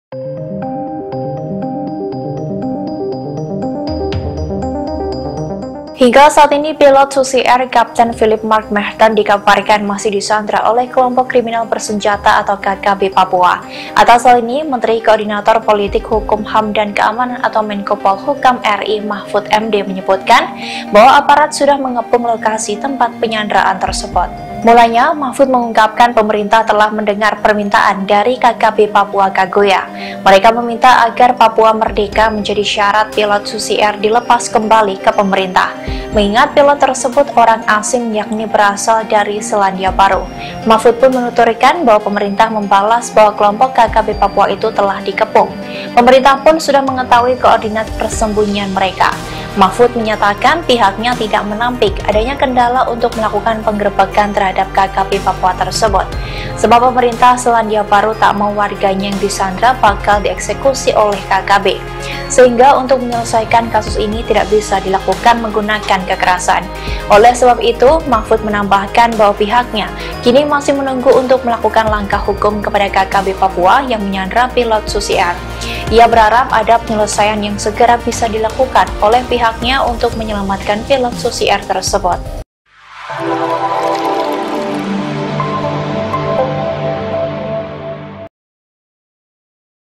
Mahfud MD Sebut Kelompok KKB Papua yang Sandera Pilot Susi Air Telah Dikepung
Hingga saat ini, Pilot Susi Air Kapten Philip Mark Mehrten dikabarkan masih disandera oleh Kelompok Kriminal Bersenjata (KKB) Papua.

Atas hal ini, Menteri Koordinator Politik, Hukum, HAM, dan Keamanan (Menkopolhukam) RI Mahfud MD menyebutkan bahwa aparat sudah mengepung lokasi tempat penyanderaan tersebut.

Mulanya, Mahfud MD mengungkapkan pemerintah telah mendengar permintaan dari KKB Papua Kagoya.